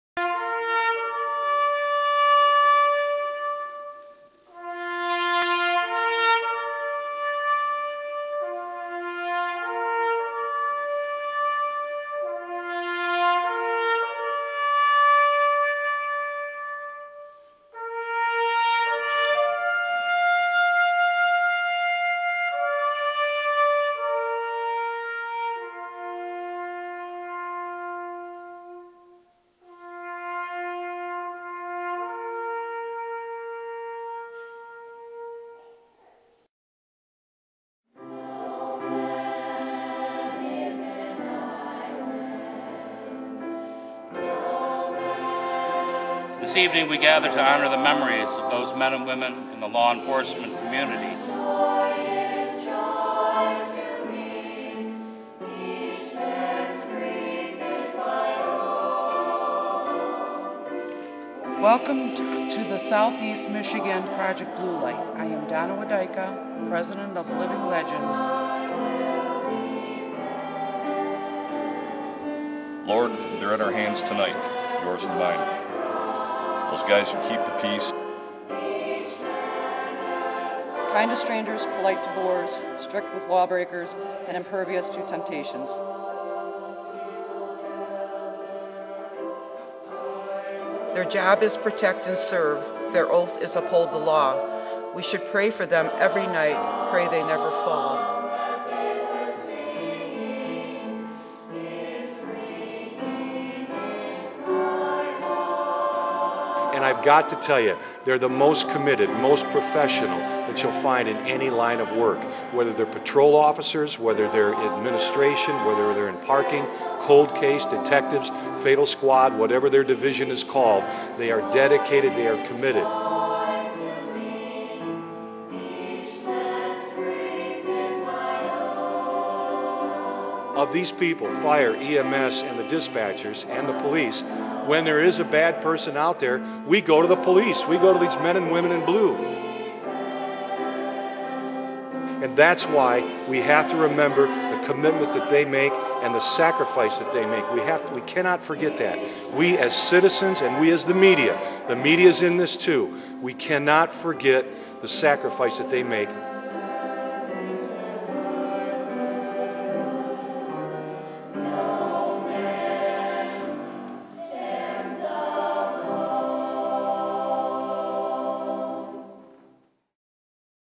Project Blue Light 2009
Living Legends hosted the 2009 Southeastern Michigan Project Blue Light on November 30th at the St. Justin Catholic Church, Hazel Park. Speakers included Living Legends President Donna Wudyka and Fox 2 News Anchor Ron Savage.  The event paid tribute to police officers and the sacifices they make to protect their communities.